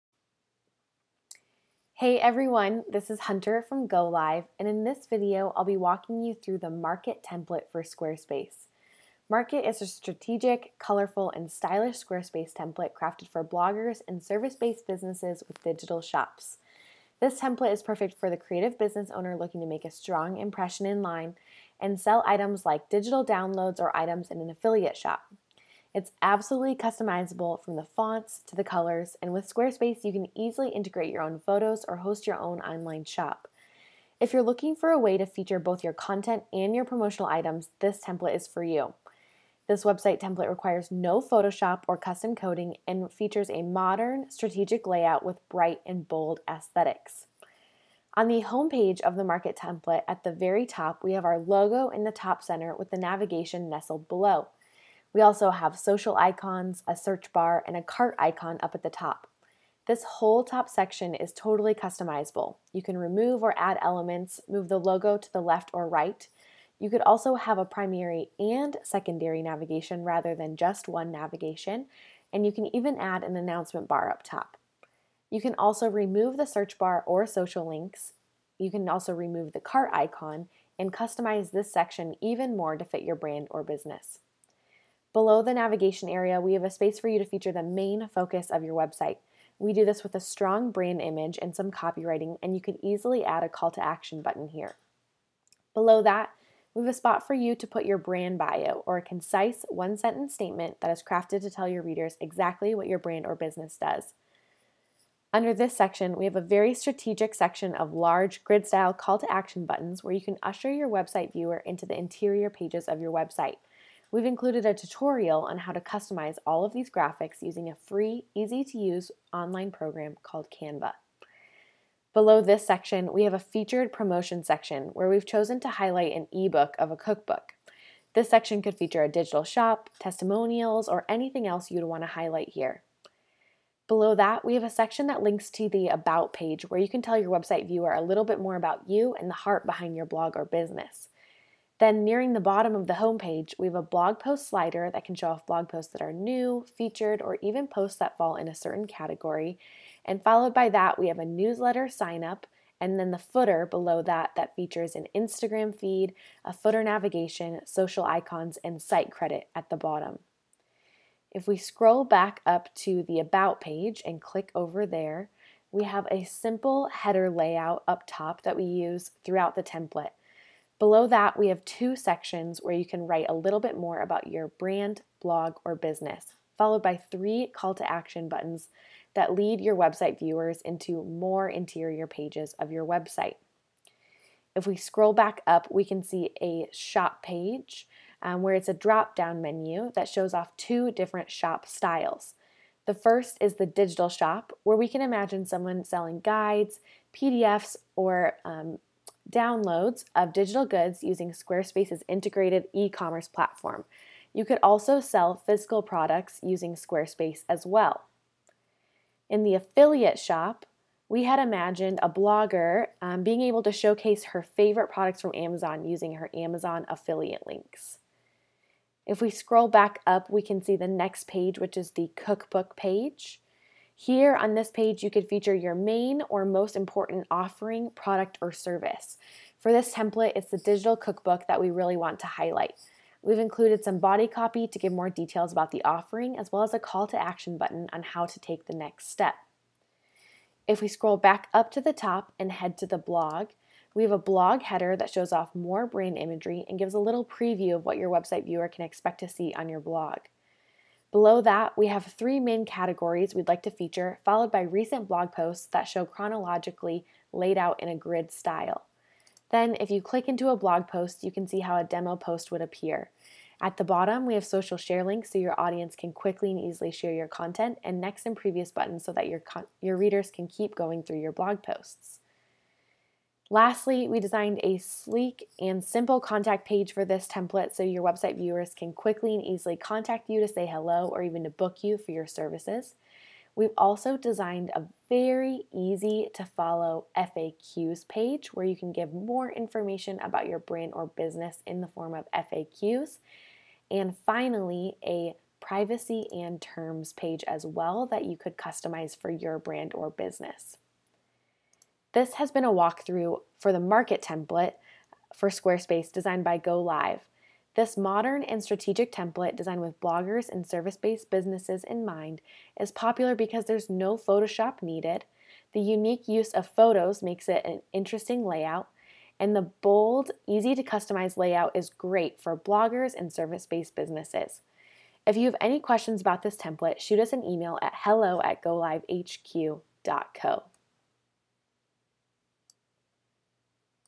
Market Walkthrough Video
Market is a strategic, colorful and stylish Squarespace Template crafted for bloggers and service-based businesses with digital shops.

It’s ABSOLUTELY customizable from the fonts, to the colors. With Squarespace, you can easily integrate your own photos, host your own online shop, and display a gorgeous portfolio of your work. If you’re looking for a way to feature both your content and your promotional items, this template is for you.